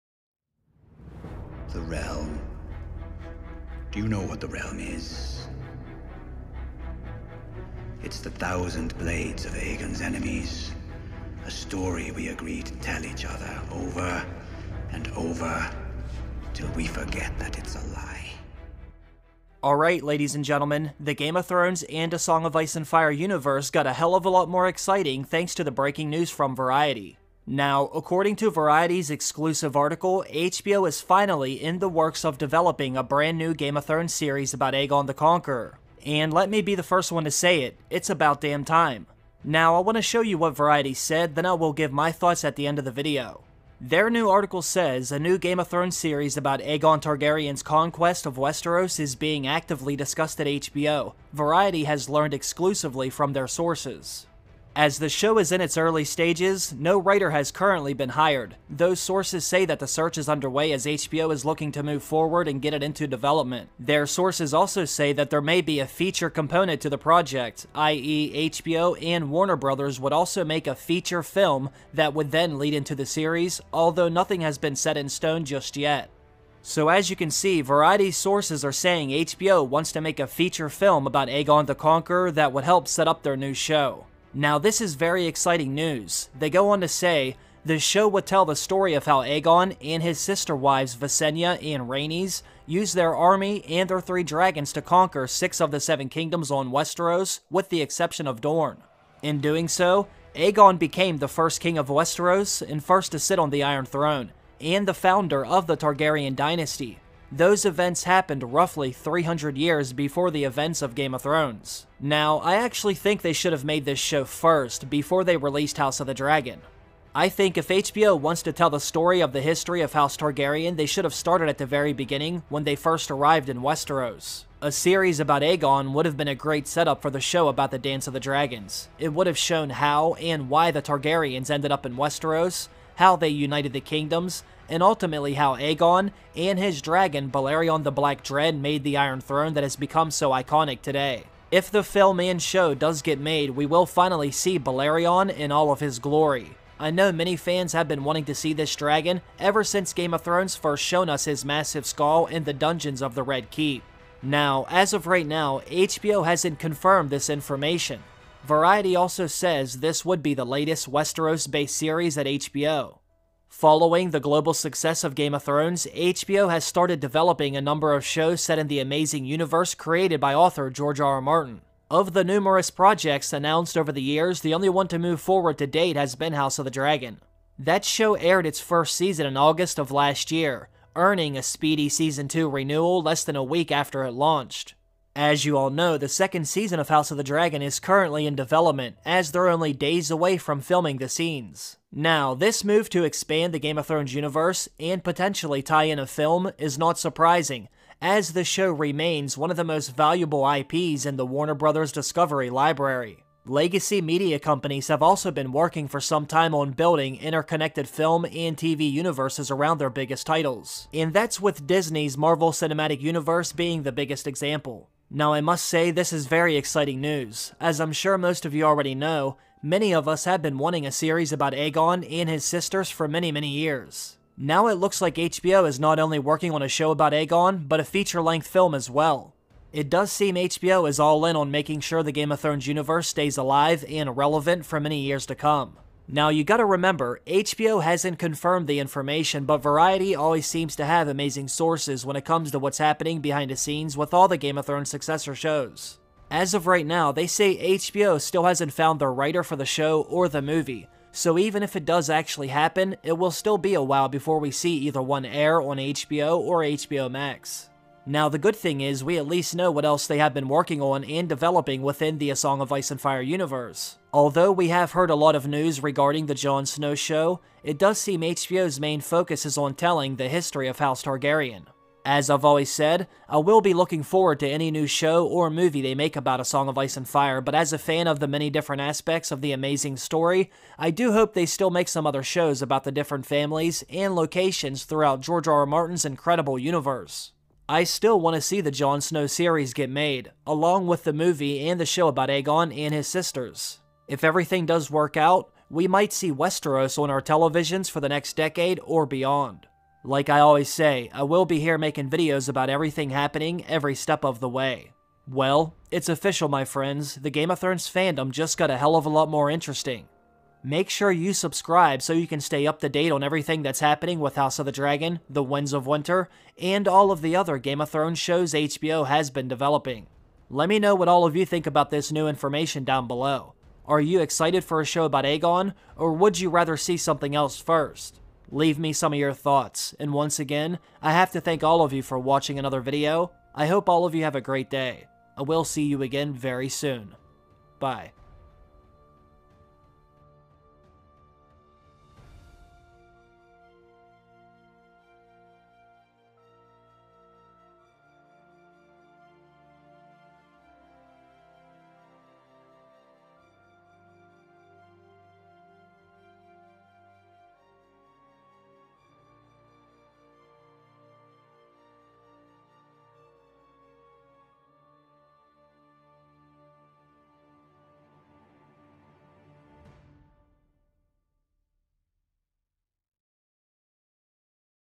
HBO EXCLUSIVE | New Game of Thrones Prequel Series & Movie About Aegon the Conqueror!
Welcome back for another Game of Thrones and House of the Dragon video. In this video, I will be talking about Variety's new article where they revealed HBO is developing a new Game of Thrones Prequel Series and movie about Aegon the Conquerer and his sister-wives! A new “Game of Thrones” prequel about Aegon I Targaryen’s conquest of Westeros is being actively discussed at HBO, Variety has learned exclusively from sources.